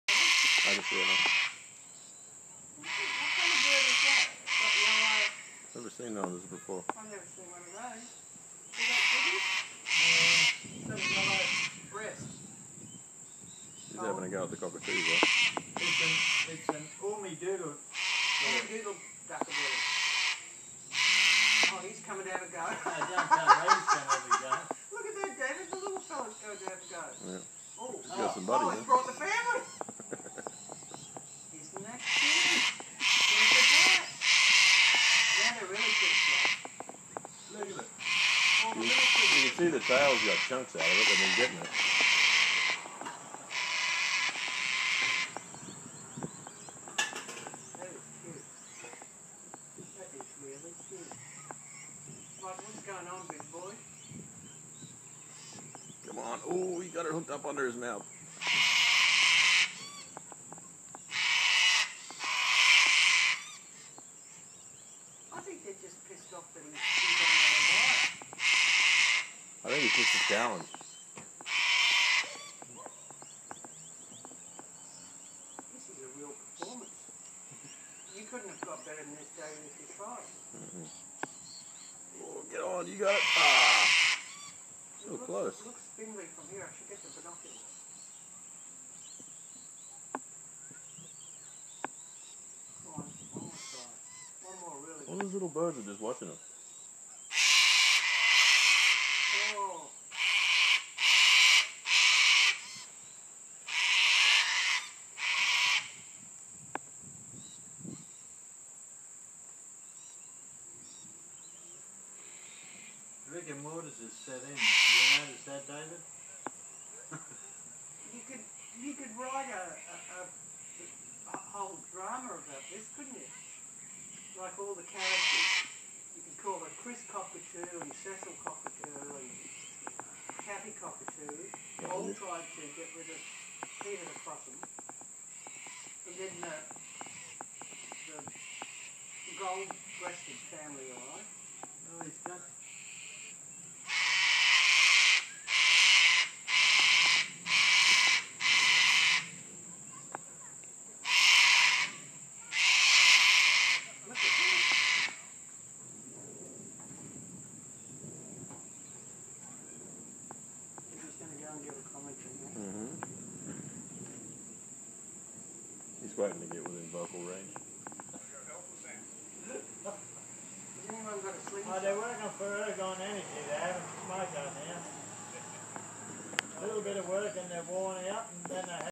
Cockatoo Cleaning Crew
Cockatoos attempt to remove an unfortunate ringtail possum from "their" power line. The dead possum died while still attached at the tail to the cable.